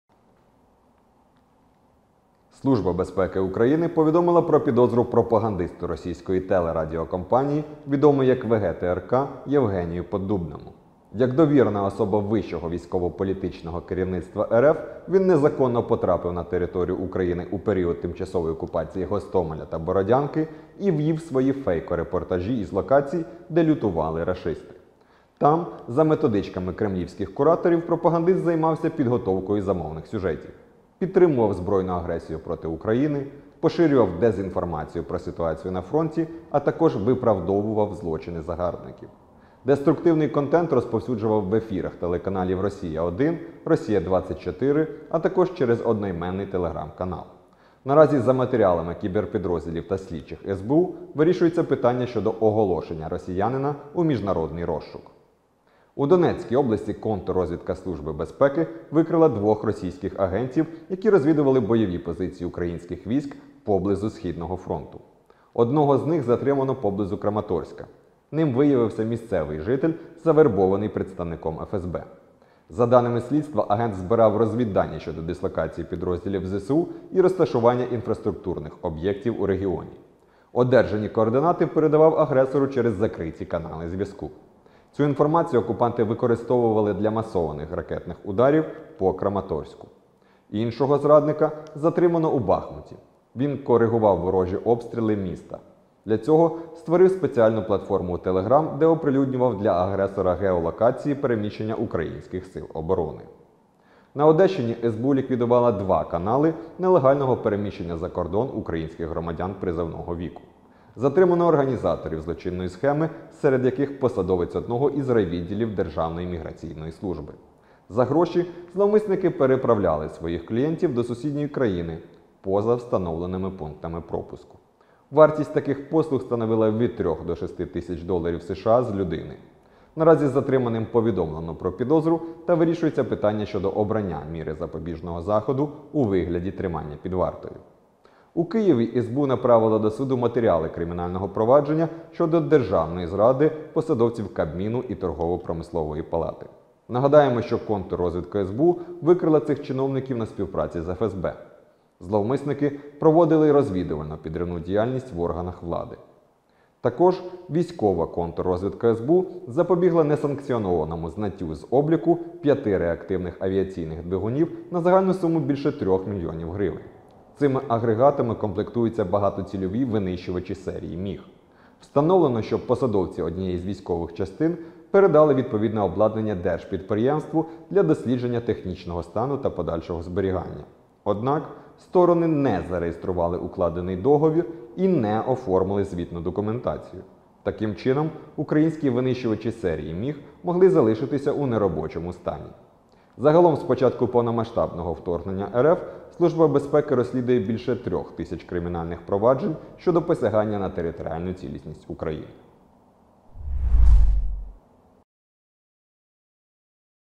СБУ затримала коригувальників ударів по Бахмуту і Краматорську та повідомила підозру пропагандисту
Служба безпеки України повідомила про підозру пропагандисту російської телерадіокомпанії (відомої як ВГТРК) Євгєнію Поддубному. Як довірена особа вищого військово-політичного керівництва рф, він незаконно потрапив на територію України у період тимчасової окупації Гостомеля та Бородянки і вів свої фейко-репортажі із локацій, де лютували рашисти.

Там за «методичками» кремлівських кураторів пропагандист займався підготовкою замовних сюжетів: підтримував збройну агресію проти України, поширював дезінформацію про ситуацію на фронті, виправдовував злочини загарбників.

Деструктивний контент розповсюджував в ефірах телеканалів «россия-1», «россия-24», а також через однойменний Телеграм-канал.

Наразі за матеріалами кіберпідрозділів та слідчих СБУ вирішується питання щодо оголошення росіянина у міжнародний розшук.

У Донецькій області:

контррозвідка Служби безпеки викрила двох російських агентів, які розвідували бойові позиції українських військ поблизу східного фронту.

Одного з них затримано поблизу Краматорська. Ним виявився місцевий житель, завербований представником фсб.

За даними слідства, агент збирав розвіддані щодо дислокації підрозділів ЗСУ і розташування інфраструктурних об’єктів в регіоні. Одержані координати передавав агресору через закриті канали зв’язку.
Цю інформацію окупанти використовували для масованих ракетних ударів по Краматорську.

Іншого зрадника затримано у Бахмуті. Він коригував ворожі обстріли міста. Для цього створив спеціалізовану платформу у Телеграм, де оприлюднював для агресора геолокації переміщення українських Сил оборони.

На Одещині:
СБУ ліквідувала два канали нелегального переміщення за кордон українських громадян призовного віку. Затримано організаторів злочинної схеми, серед яких – посадовець одного з райвідділів Державної міграційної служби.

За гроші зловмисники переправляли своїх клієнтів до сусідньої країни поза встановленими пунктами пропуску. Вартість таких «послуг» становила від $3 до $6 тис. з людини.

Наразі затриманим повідомлено про підозру та вирішується питання щодо обрання міри запобіжного заходу у вигляді тримання під вартою.

У Києві:
СБУ направила до суду матеріали кримінального провадження щодо держзради двох посадовців Кабміну і Торгово-промислової палати.

Нагадаємо, що у червні цього року контррозвідка викрила цих чиновників на співпраці з фсб. Зловмисники проводили розвідувально-підривну діяльність в органах влади.

Також військова контррозвідка СБУ запобігла несанкціонованому зняттю з обліку п’яти реактивних авіаційних двигунів на загальну суму більше 3 млн грн. Цими агрегатами комплектуються багатоцільові винищувачі серії МіГ.

Встановлено, що посадовці однієї з військових частин передали відповідне обладнання держпідприємству для дослідження технічного стану та подальшого зберігання.

Однак сторони не зареєстрували укладений договір і не оформили звітну документацію. Таким чином, українські винищувачі серії МіГ могли залишитися у неробочому стані.

Загалом з початку повномасштабного вторгнення рф Служба безпеки розслідує більше 3 тис. кримінальних проваджень щодо посягання на територіальну цілісність України.